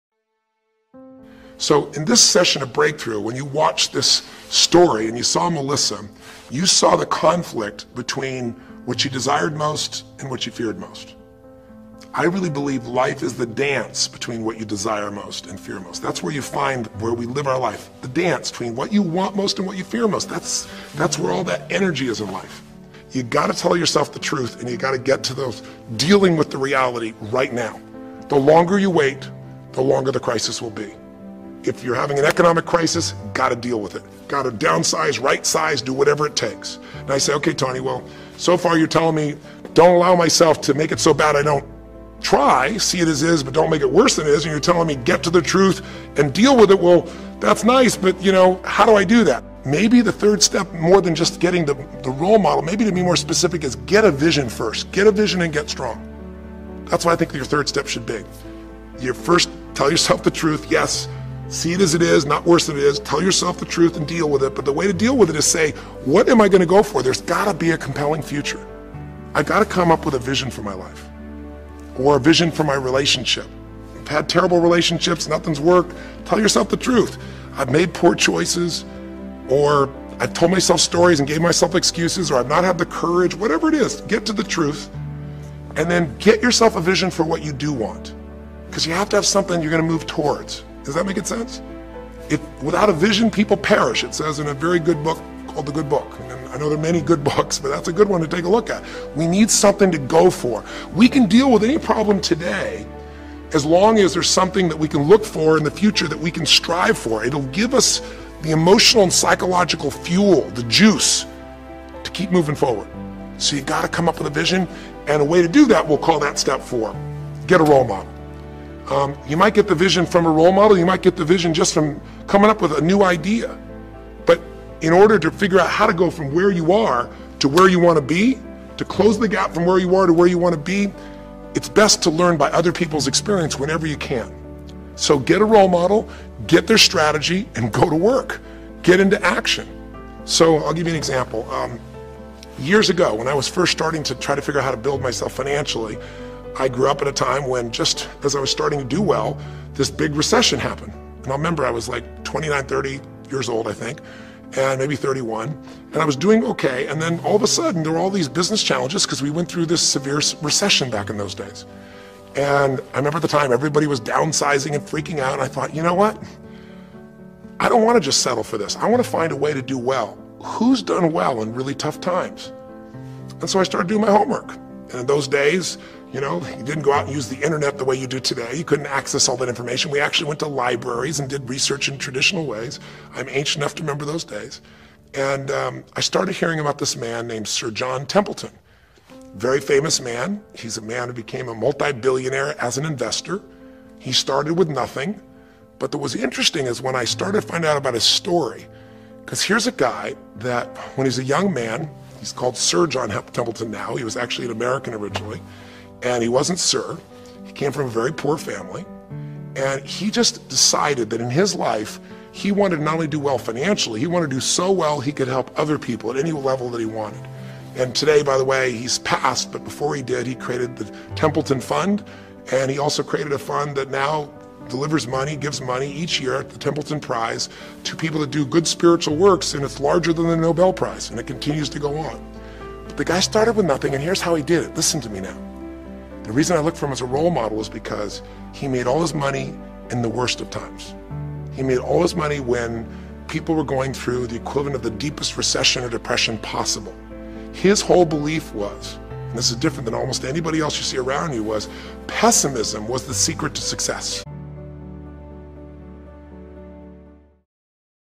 Tony Robbins | Life is What you Desire and Fear Most | Motivational
" Life is the dance between what you desire most and fear most" ~ Tony Robbins

Stop procrastinating.  Make a plan for yourself and stick to it, don't let fear stop you.  "The longer you wait, the longer the crisis will be".  Successful individuals visualize what they want, have a positive mindset and take action on their goals.   We can be our own worst enemy, but we have to stop the self sabotage.  Peace & Love to All watching~ have an awesome day,  and don't let anyone bring you down!  Let's get it!

Video Footage By:  Ruvim Miksanskiy • Portland, USA 🙌